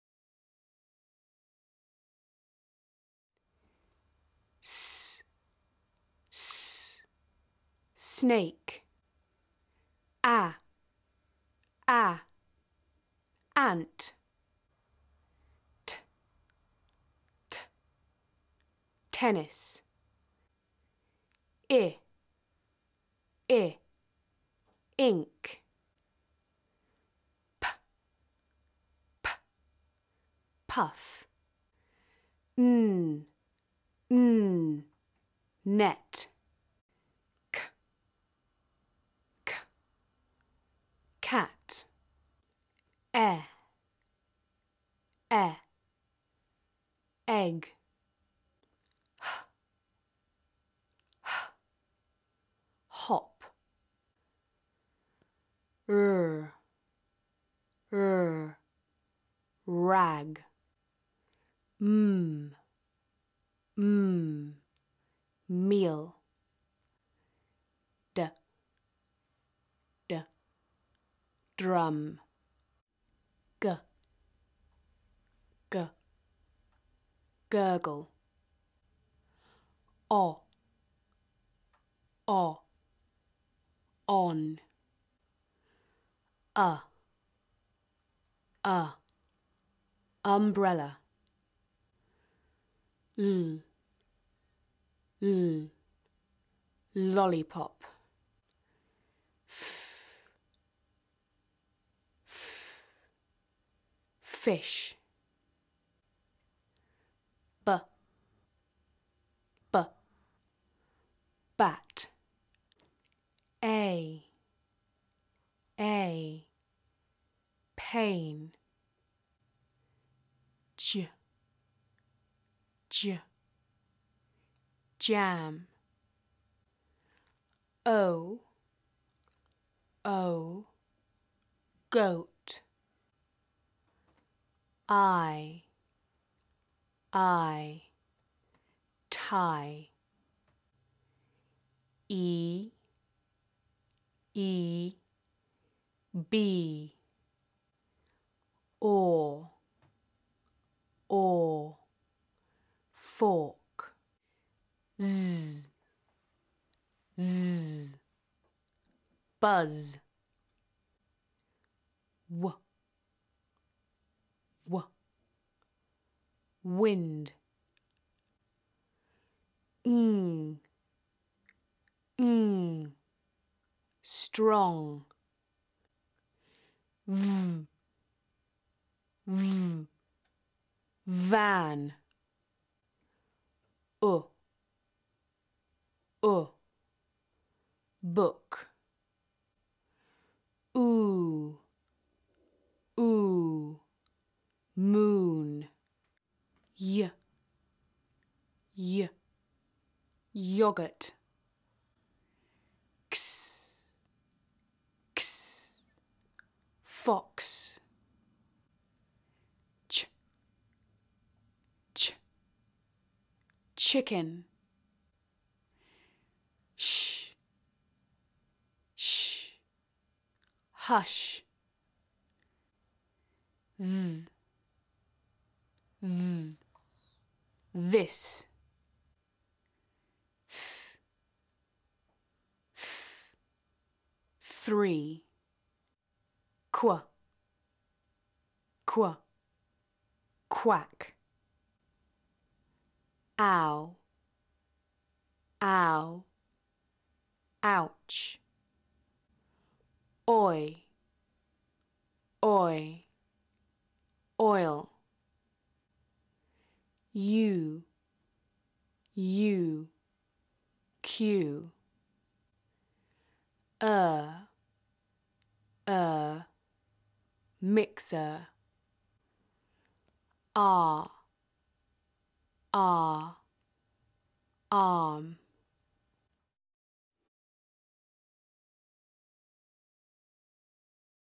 Jolly Phonics Letter Sounds (British English)
Listen to the 42 letter sounds of Jolly Phonics, spoken in British English. Each letter sound is clearly spoken twice, before an example word is given.

Perfect for parents, teachers and children who aren't sure how to pronounce the letter sounds.